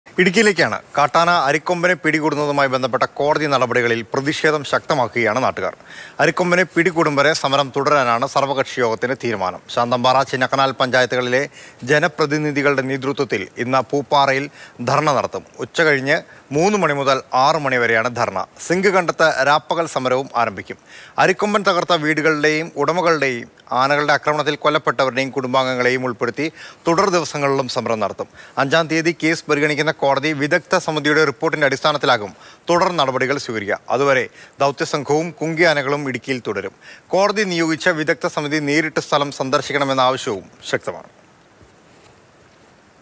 മിഷൻ അരിക്കൊമ്പന് തടയിട്ട കോടതി നടപടിയിൽ പ്രതിഷേധം ശക്തമാക്കി നാട്ടുകാർ | mission arikkomban
അരിക്കൊമ്പനെ പിടികൂടും വരെ സമരം തുടരാനാണ് സർവ്വകക്ഷി യോഗ തീരുമാനം.

Happy viewing!